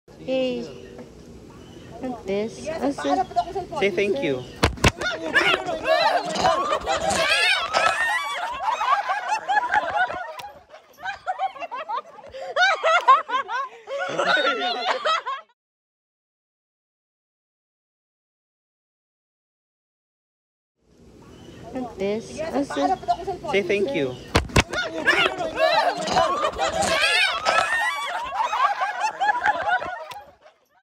Monkey at zoo swipes phone from visitor's hands, records her reaction
A monkey at a zoo saw an opportunity and swiped a cell phone from a visitor's hands, flipping it around to record her reaction.